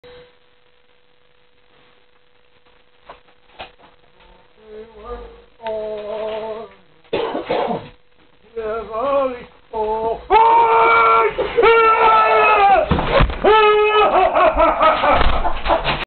FUNNIEST VIDEO EVER!
Grown Man VERY scared of rats
funny joke!! it will make you cry laughing it's VERY FUNNY.
Dad sings and then sees the rat on the step, takes one look and screams in terror!